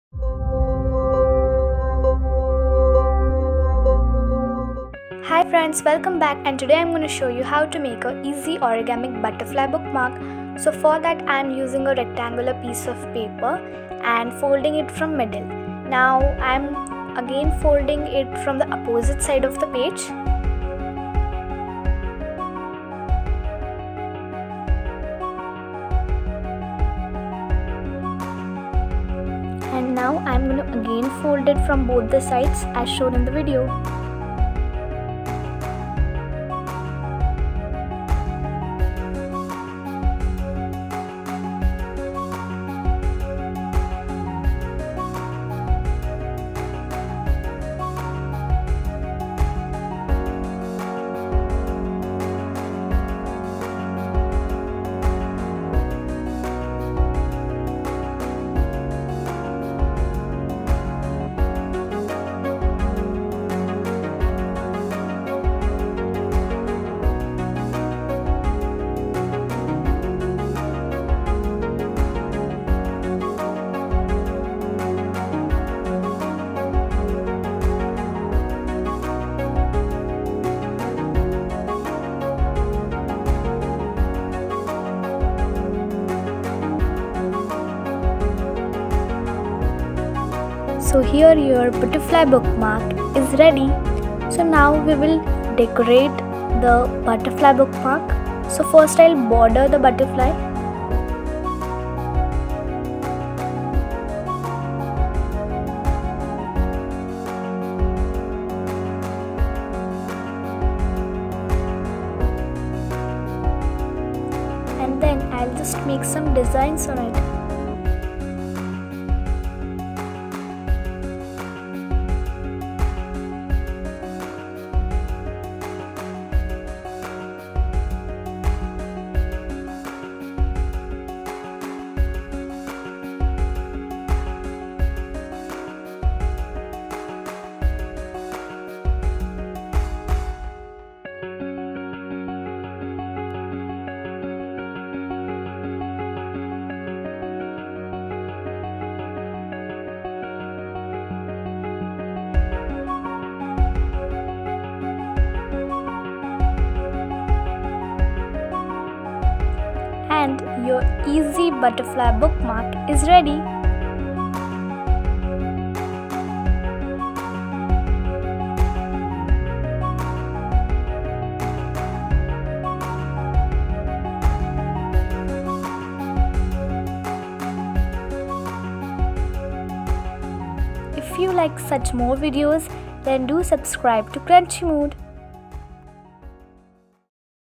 ORAGAMI BUTTERFLY BOOKMARK/ORAGAMIC PAPER CRAFT/BOOKMARK IDEAS/HOW TO MAKE BOOK MARK
Hi friends,
In this video i will show you how to make a Easy Butterfly bookmark i hope you will like it❤️😊